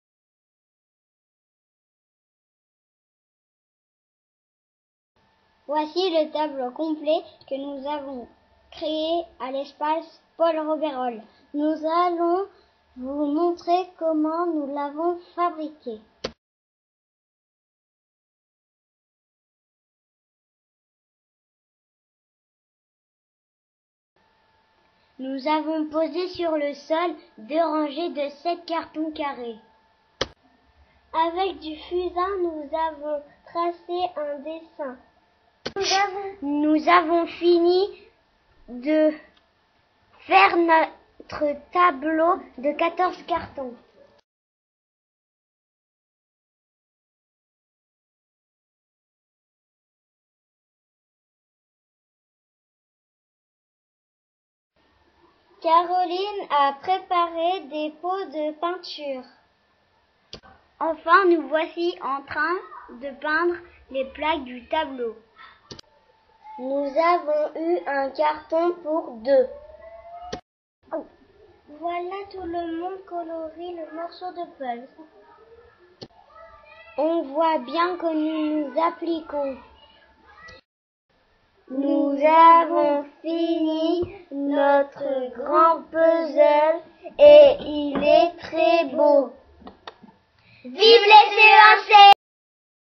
le tableau d'Eymoutiers
Création d'une fresque au centre Paul Rebeyrolle d'Eymoutiers